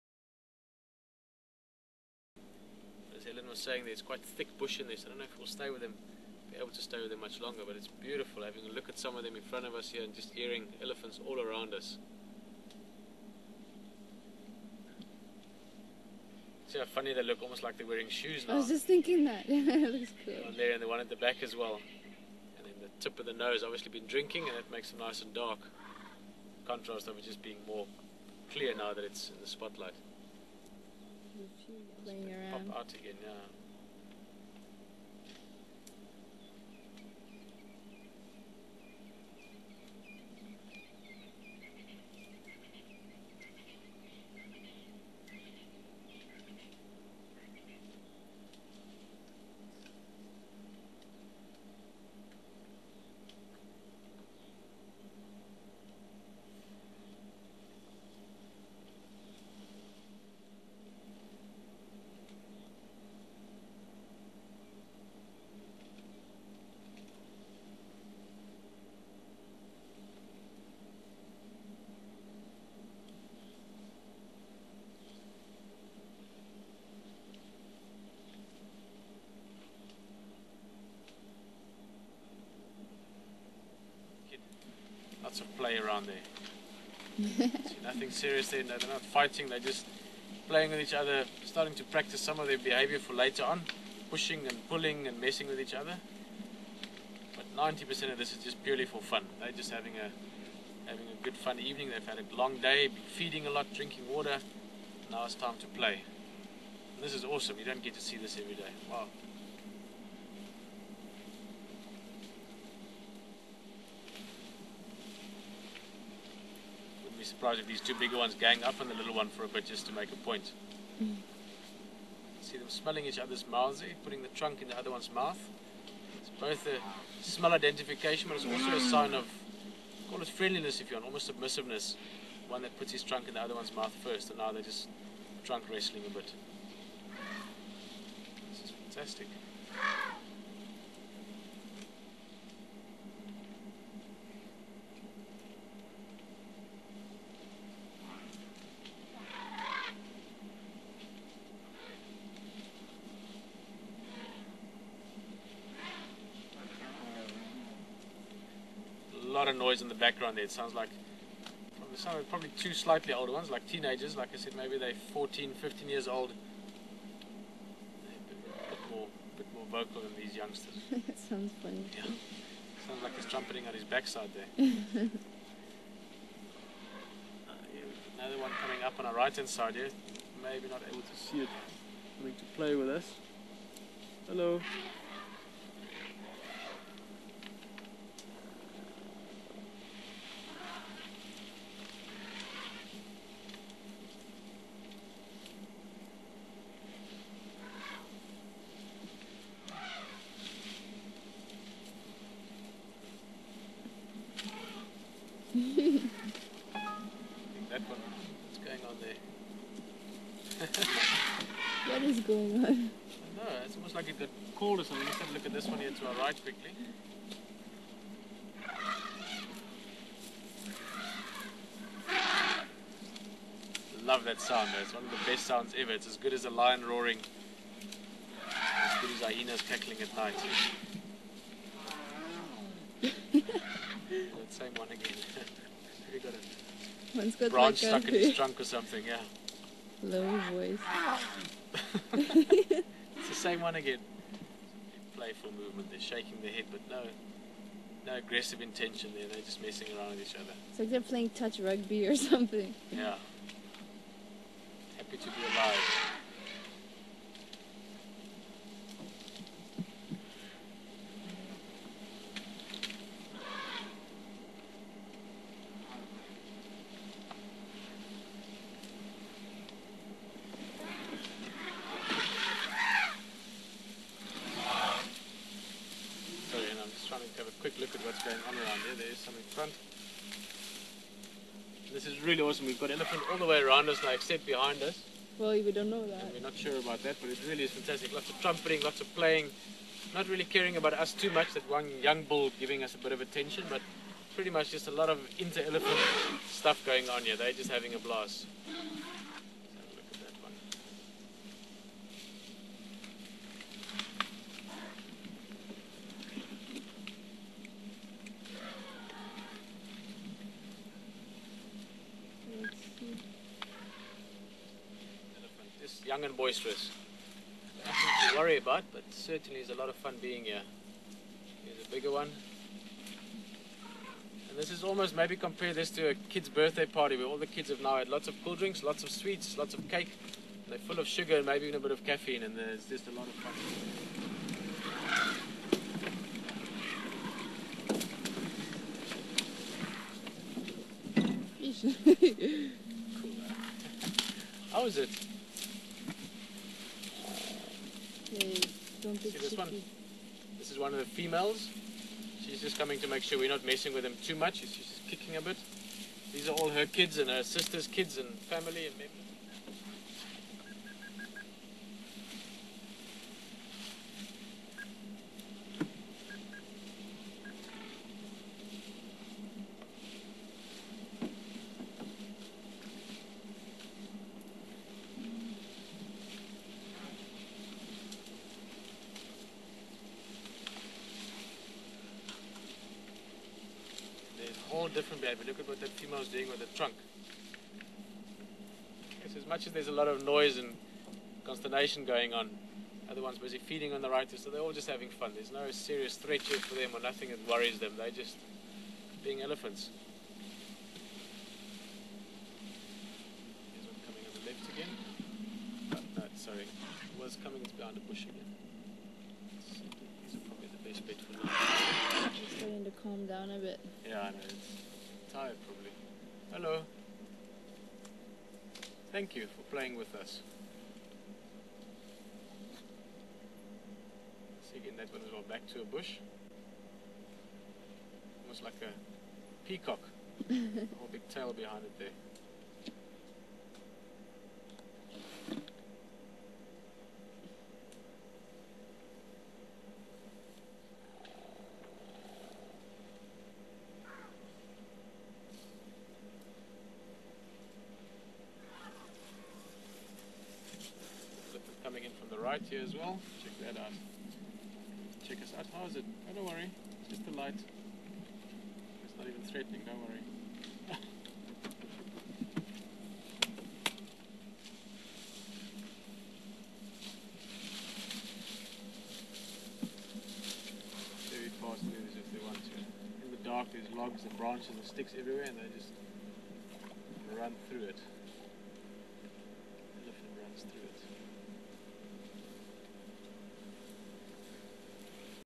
Playful Ellies
Pieter & Helen surrounded by a herd of playful elephants.  Lots of noise & trunk waving.  And some tense moments, at least for me!